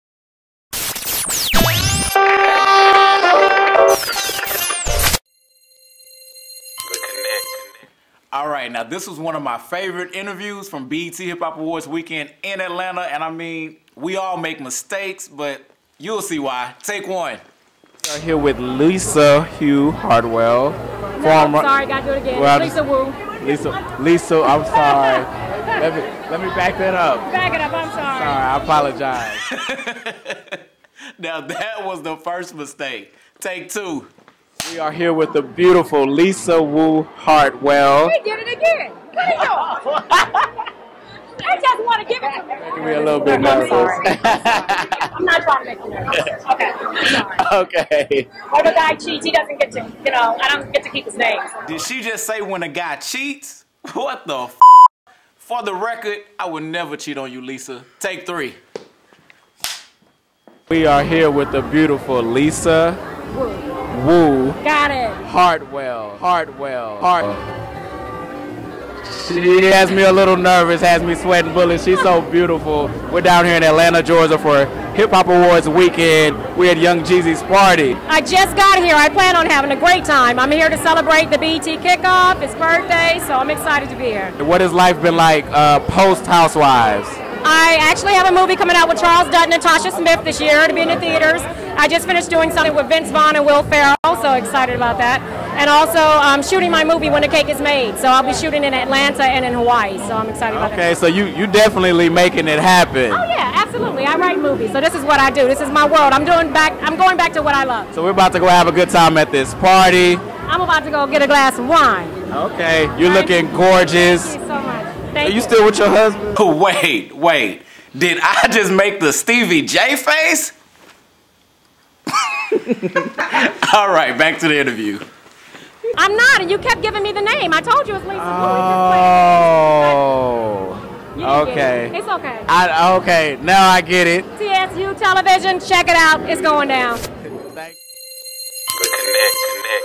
The Connect: Lisa Wu discusses upcoming projects, laughs off name mishap
Host Kenneth Ware Jr. (@KennethWareJr) caught up with former Real Housewives of Atlanta star Lisa Wu (@1LisaWu) during the 2012 BET Hip Hop Awards' weekend in Atlanta at Young Jeezy's "Evening of Luxury" gala.